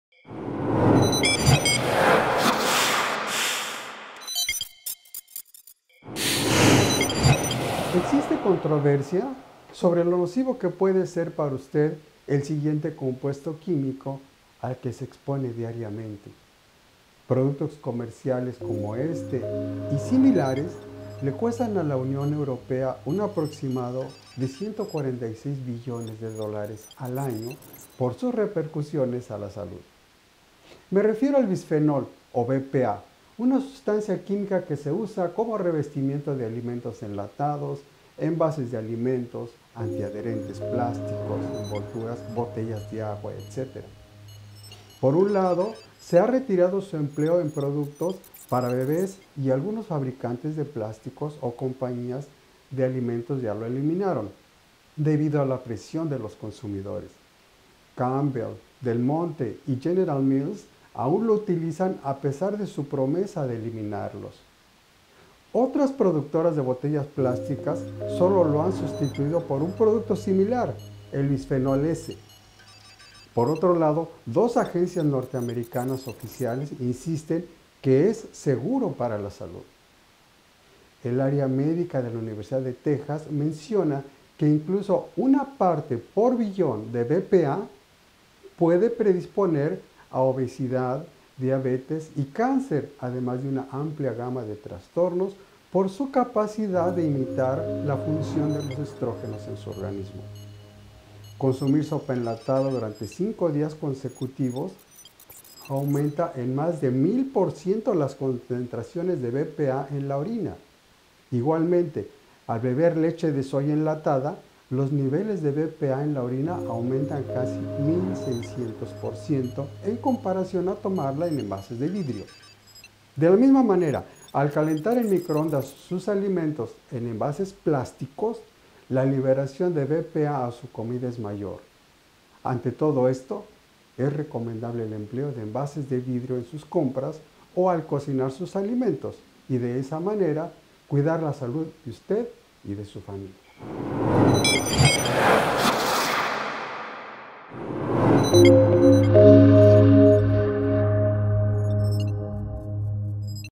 Bisfenol-A (BPA) sustancia química que se usa en alimentos Salud Total DR. Ricardo Ramos
Existe controversia sobre sobre lo nocivo que puede ser para
usted el siguiente compuesto quimico al que se expone
diariamente. Productos comerciales como este y similares le
cuestan a la U Europea un aproximado de $ 146 billones de
USD al año por sus repercusiones a la salud.
Me refiero al Bisfenol-A (BPA)